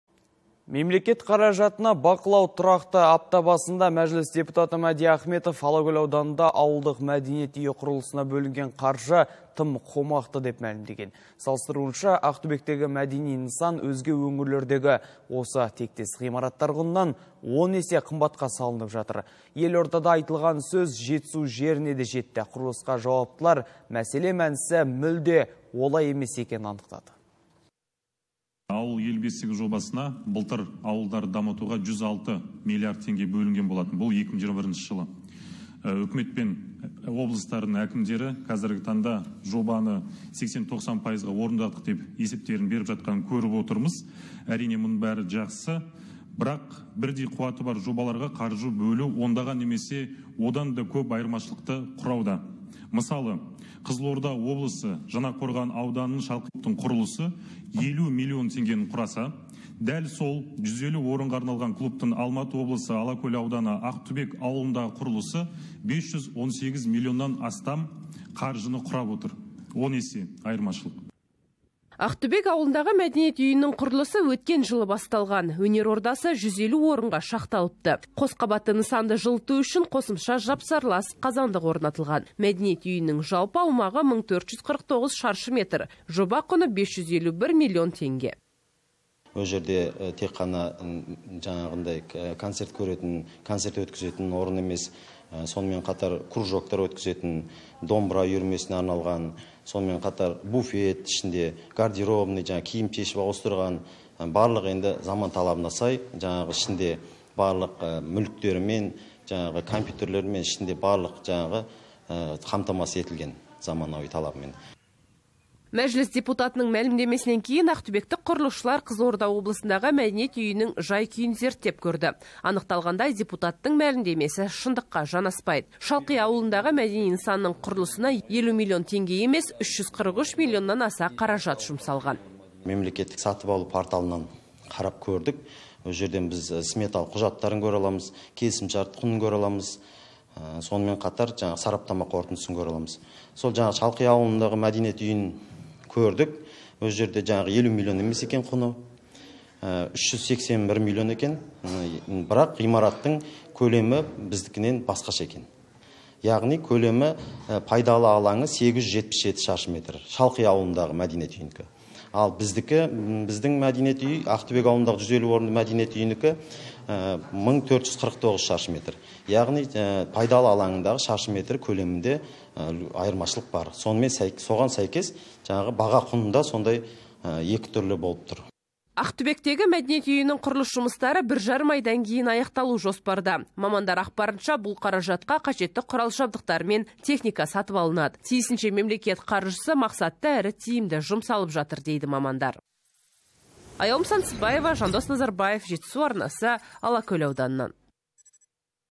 Мемлекет қаражатына бақылау тұрақты
Апта басында мәжіліс депутаты Мәди Ахметов Алакөл ауданында ауылдық мәдениет үйі құрылысына бөлінген қаржы тым қомақты деп мәлімдеген. Салыстыруынша, Ақтүбектегі мәдени нысан өзге өңірлердегі осы тектес ғимараттар құнынан 10 есе қымбатқа салынып жатыр. Елордада айтылған сөз Жетісу жеріне де жетті. Құрылысқа жауаптылар мәселе мәнісі мүлде олай емес екенін анықтады.